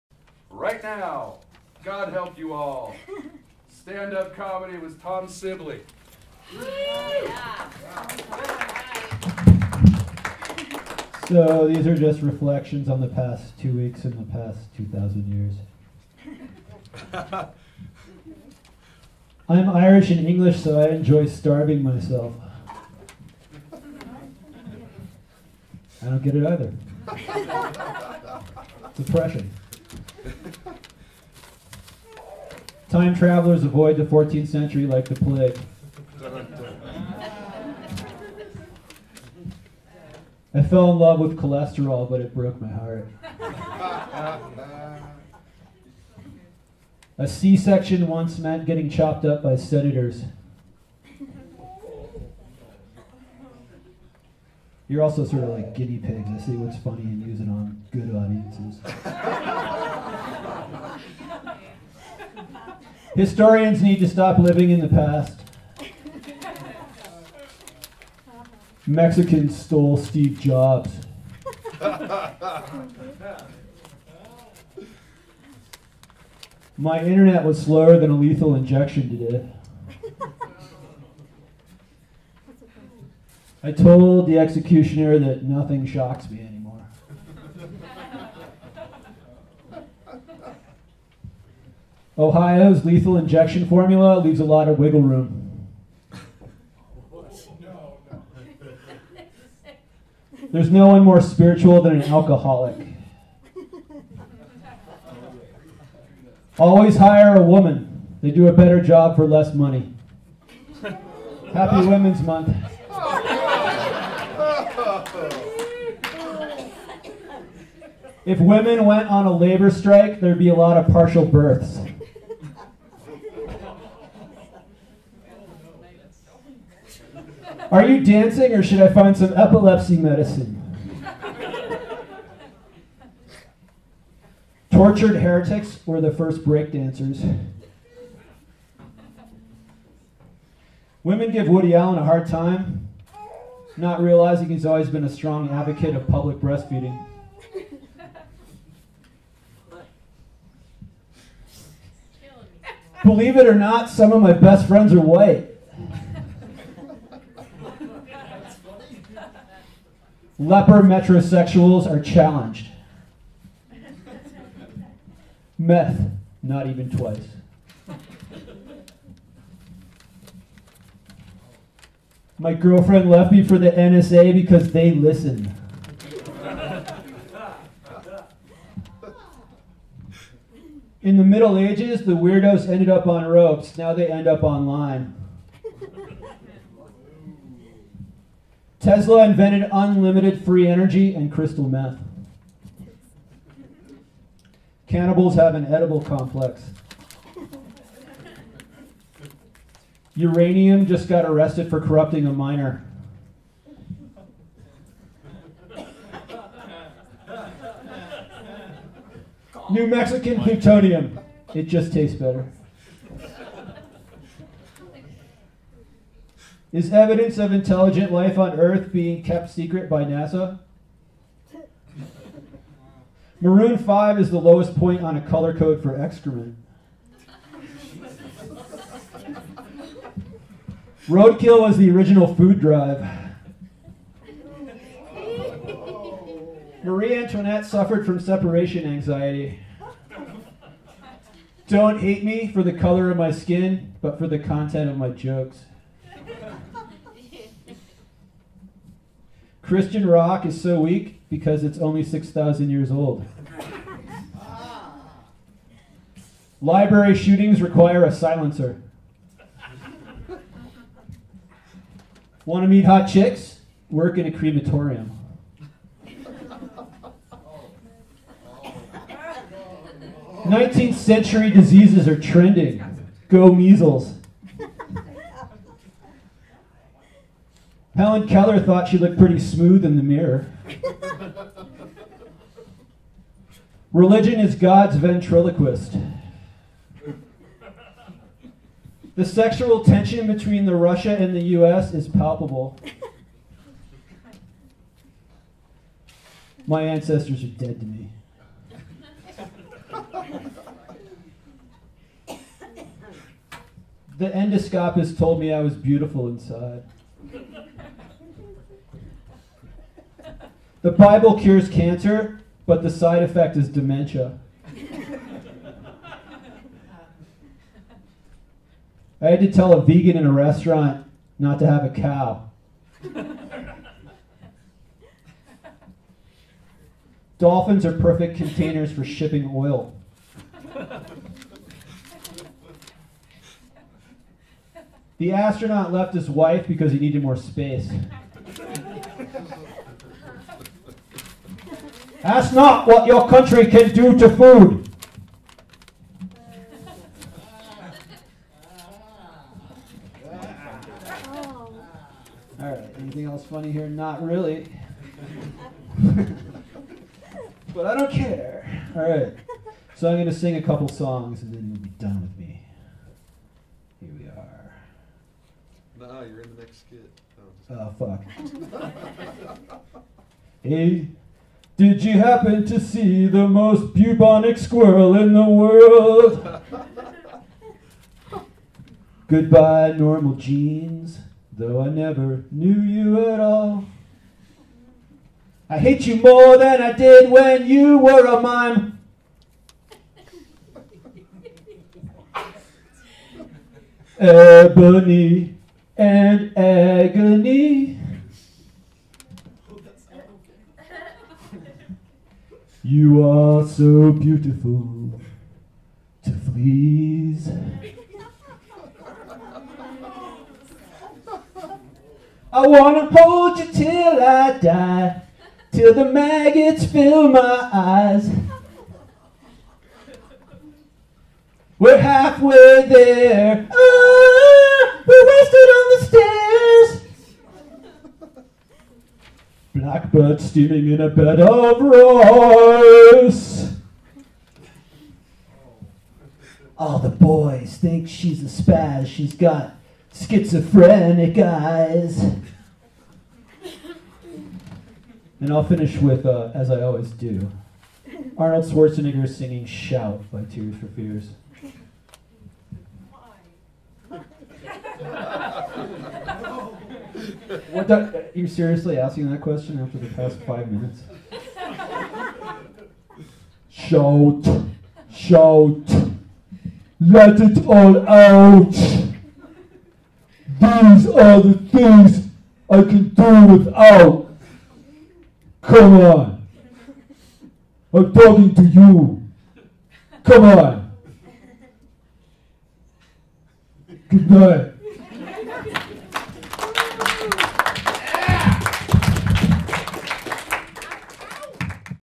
JWF24 A3S3 Stand Up comedy ~ Tom Sibley
Julesworks Follies is a long running variety show in Santa Fe NM.  This 24rd edition was held at the Jean Cocteau Theater March 31 2014.  Comedy, music, poetry, magic and dance were featured. The shows are produced by Jules Rubin.